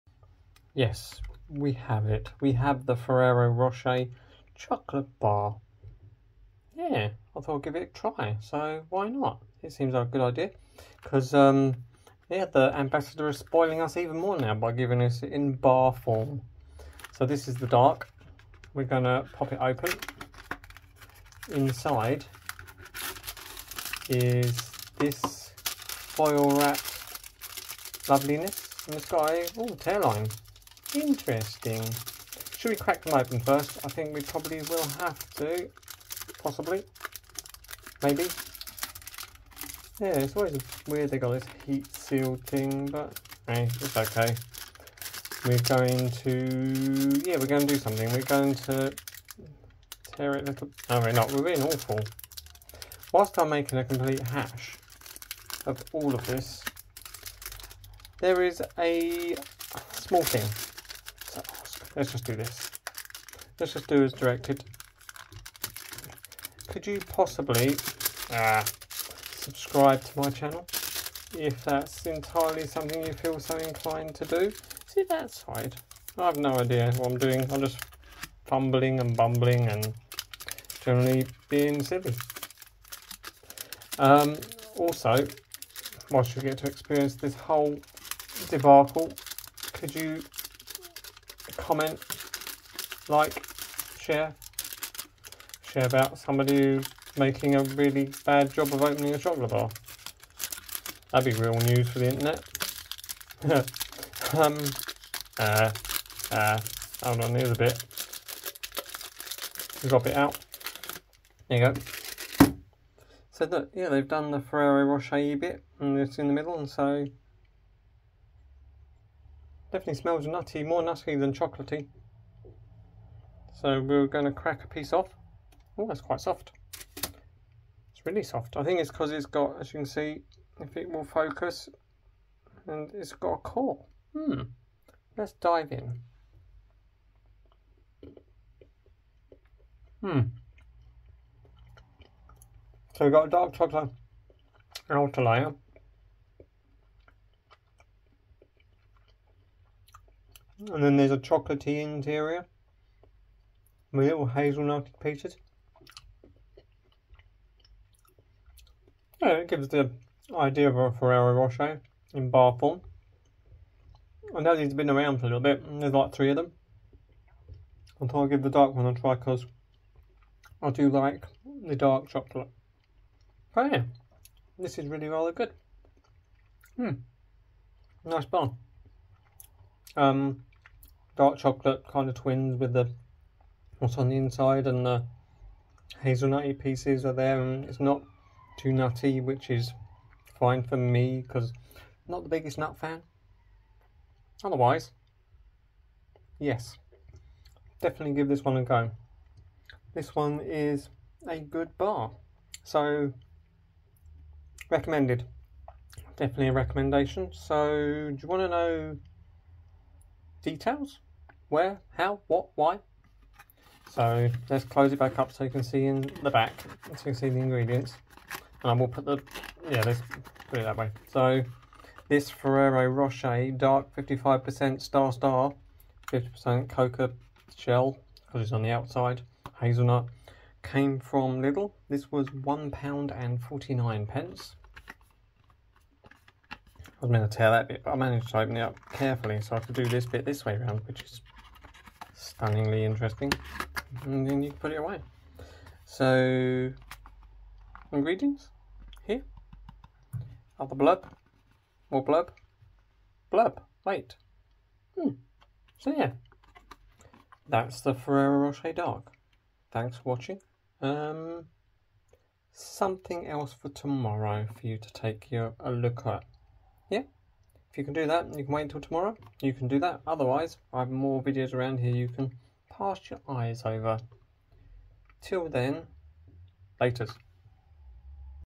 Ferrero Rocher Dark - Random Reviews
Ferrero Rocher the classic confection has now been formed into a chocolate bar, let's check out what the ambassador is spoiling us with.